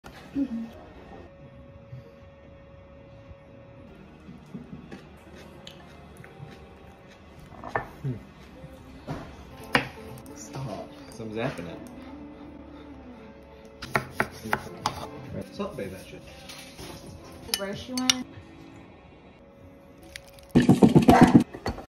Refreshing & simple Korean Cucumber Salad
A quick & easy 30 min - 1 hour Korean cucumber salad that is extremely refreshing w/ a slight hint of spice.